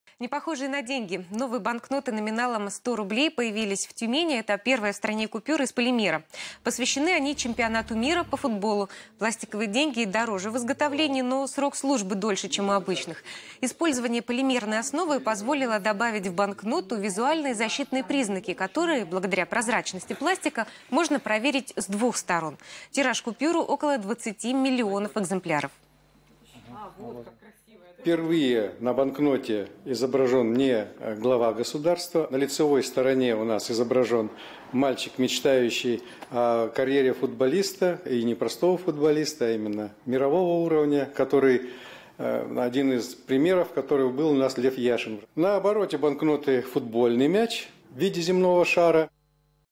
«Футбольные» деньги
Новые банкноты номиналом 100 рублей появились в Тюмени. Это первая в стране купюра из полимера. Посвящены они чемпионату мира по футболу. Пластиковые деньги дороже в изготовлении, но срок службы дольше, чем у обычных. Использование полимерной основы позволило добавить в банкноту визуальные защитные признаки, которые, благодаря прозрачности пластика, можно проверить с двух сторон. Тираж купюры около 20 миллионов экземпляров.